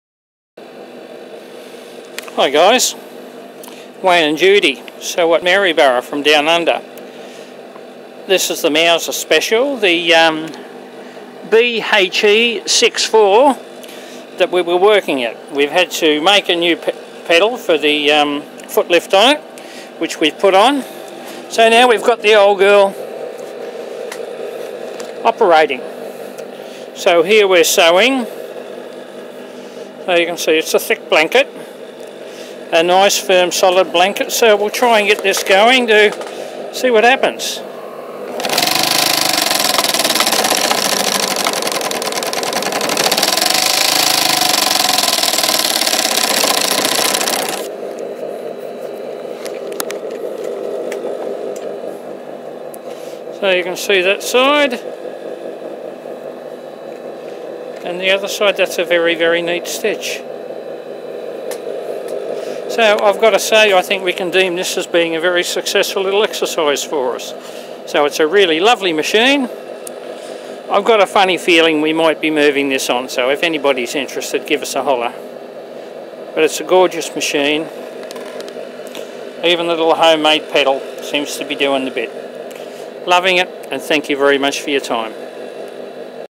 Mauser Spezial BHE 6-4
What a gem. Here sewing two thicknesses of thick heavy blanket. What a trooper.  Still has a Bit of use to free it completely up . But one amazing machine.  We plan on Moving this on  so if anyone is interested just message us.